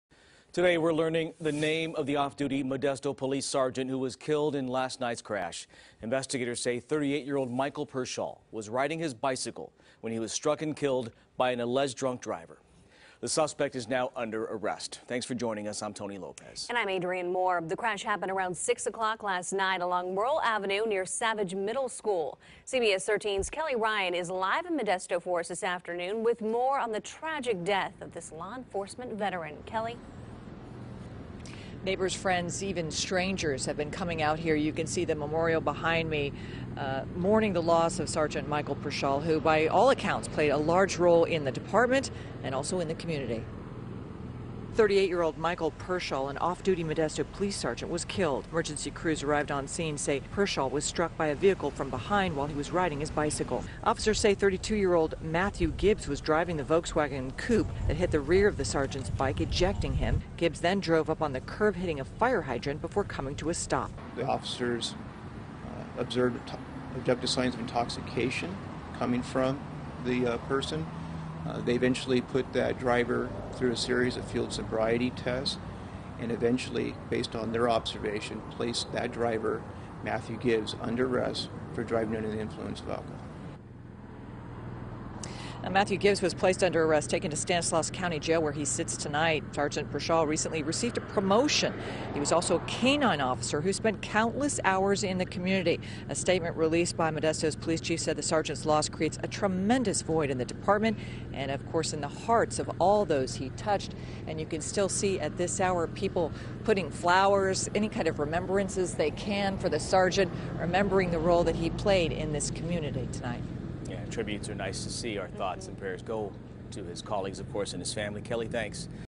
Police Identify Off-Duty Officer Killed By Suspected DUI Driver
Investigators say 38-year-old Sgt. Michael Pershall was riding his bike when he was hit from behind. The car then crashed into a fire hydrant. The suspect in the crash has been arrested.